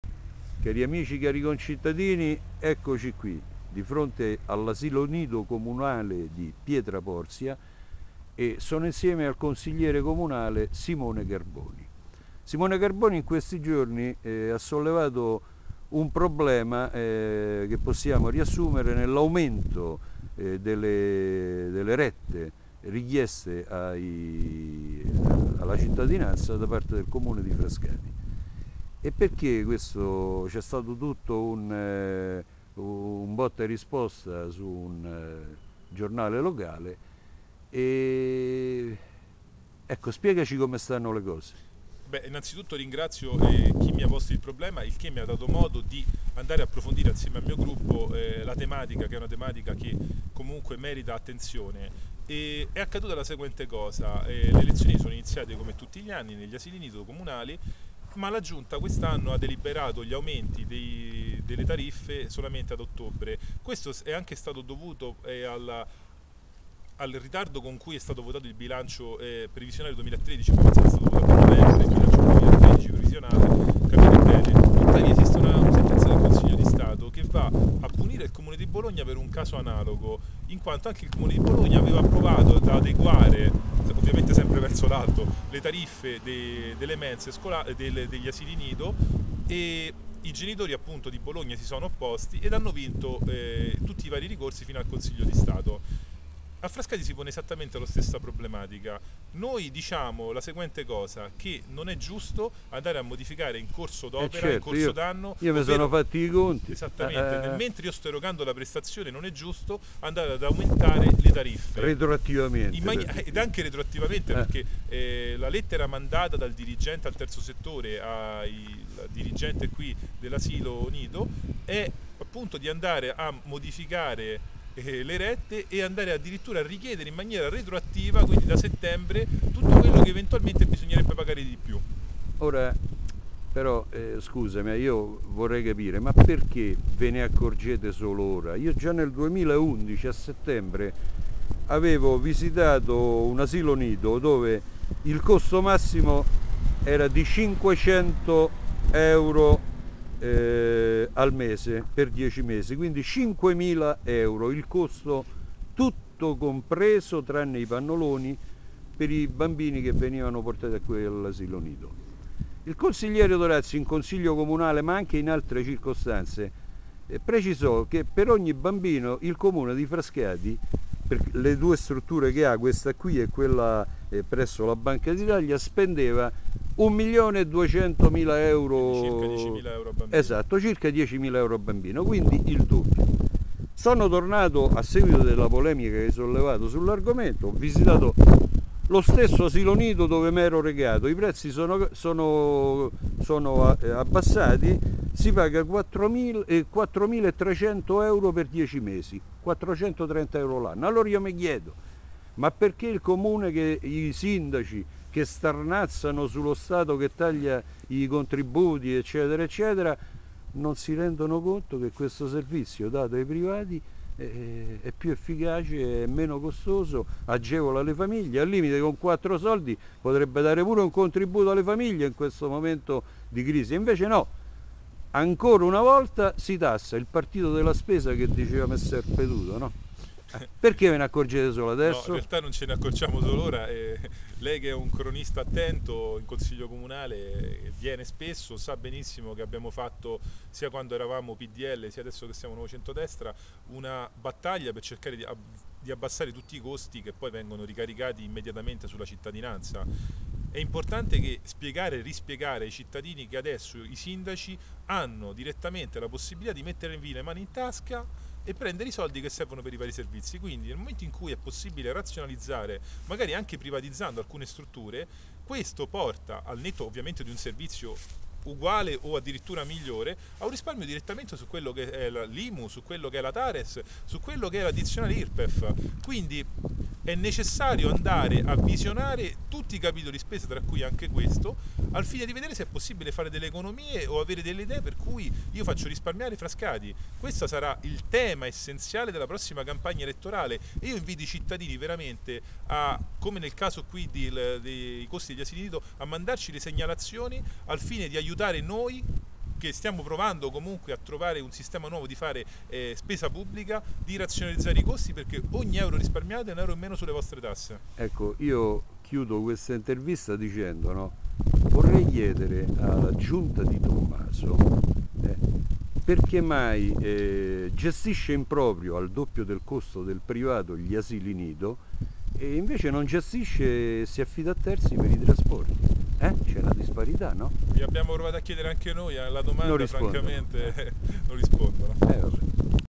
FRASCATI - SIMONE CARBONI SULL' AUMENTO DELLE RETTE ASILI NIDO
L'AUMENTO IN CORSO D'ANNO E' IL CASO DEL GIORNO. Sull'argomento, dopo aver intervistato il consigliere Sandro D' Orazio, ho avuto uno scambio d'idee direttamente con chi il problema ha posto all'attenzione della politica frascatana.